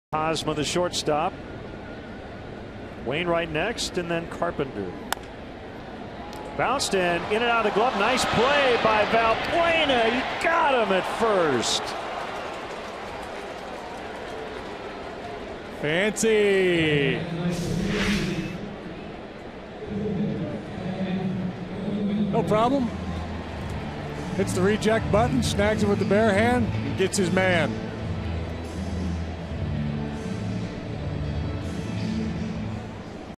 2013/06/19 Valbuena's slick play
CHC@STL: Valbuena deflects it, makes barehanded throw